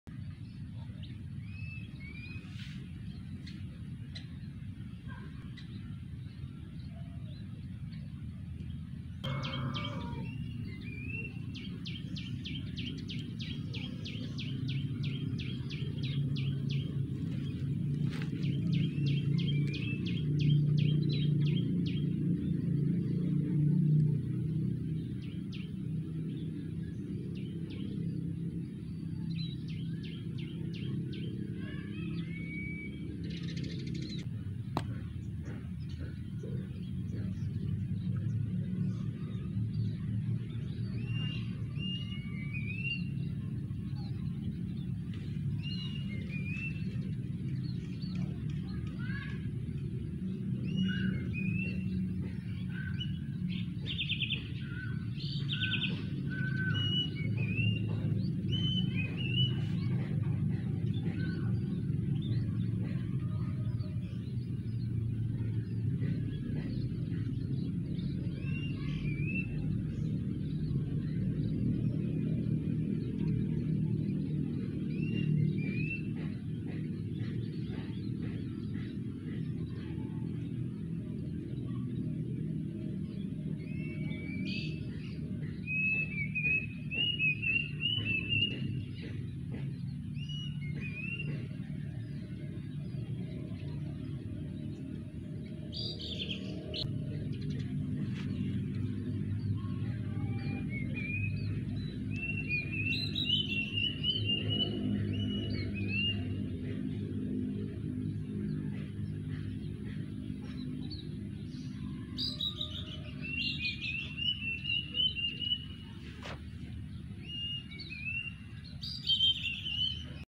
Burung Kukuk (Cuckoo) Bercuit Di Pohon
Tiga ekor burung kukuk bermain-main di pohon. Mereka saling bersaut dengan suara yang melengking.

Di desa kami burung ini biasa disebut "sirit uncuing". Warga desa sering menghubungkan burung ini dengan mitos kematian.